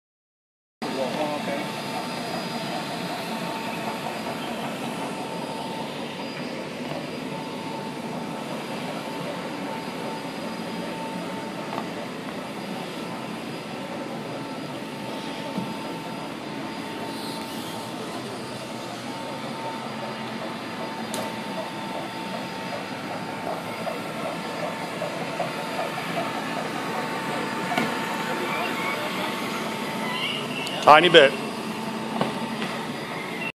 iScrub 20 on a pristine polished concrete floor
WAXIE demo with Windsor Chariot. Smooth shiny concrete.